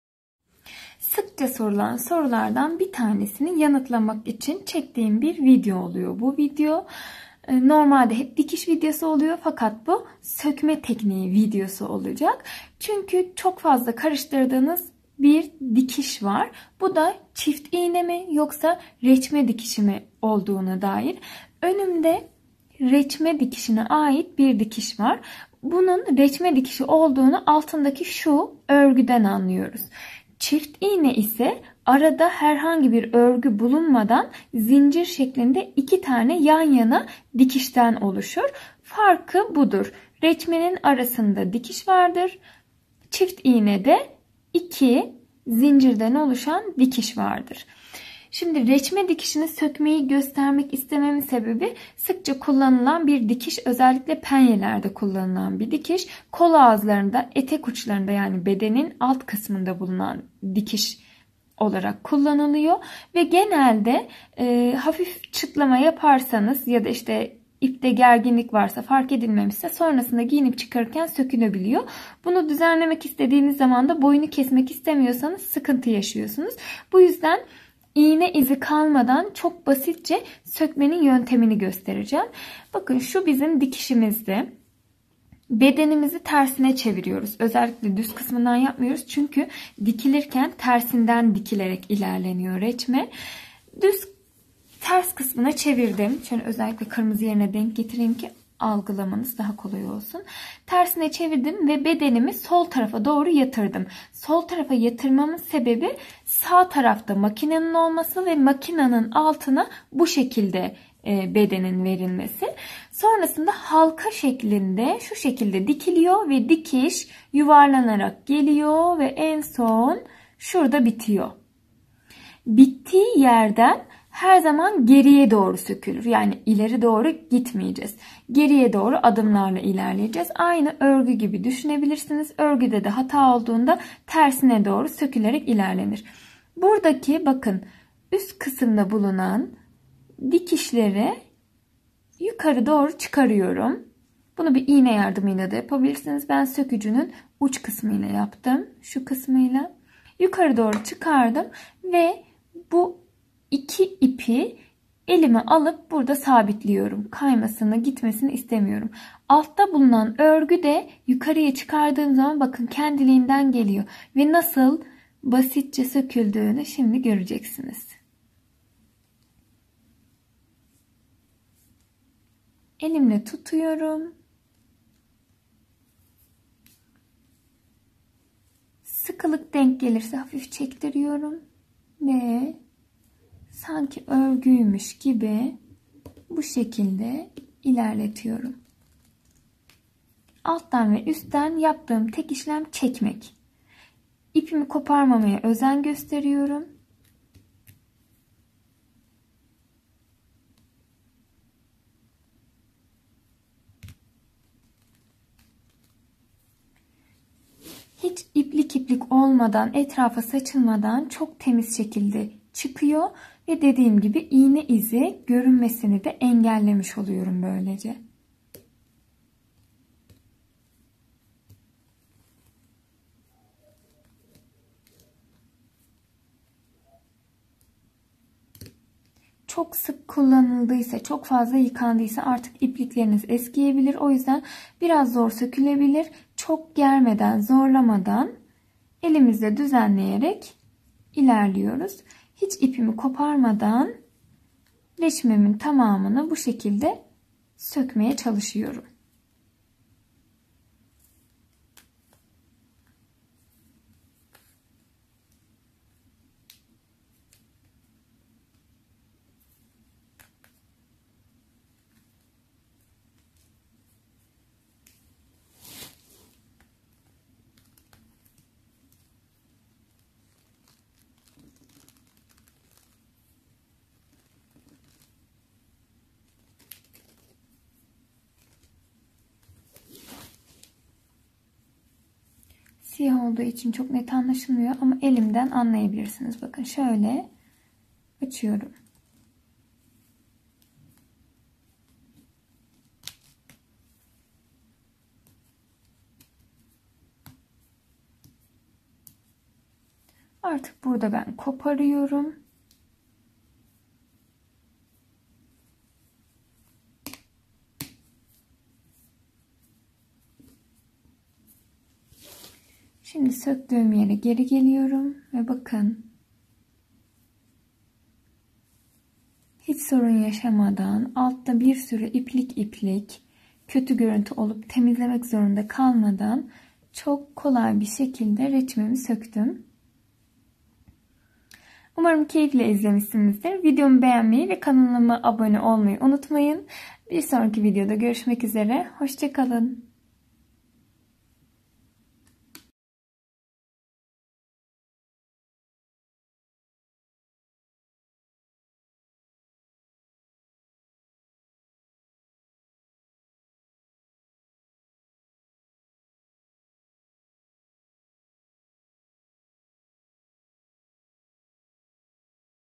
PRATİK DİKİŞ SÖKME TEKNİĞİ
Merhaba,

Bugün sizlere çift iğne ve reçme dikiş farkı nedir anlattım.
Ayrıca sizlere pratik olarak reçme dikişi nasıl sökülür gösterdim.

Öneri ve yorumlarınız benim için önemli. Bekliyorum.

Keyifli seyirler.

Daha Önceki Videolarıma Aşağıdaki Linklerden Ulaşabilirsiniz.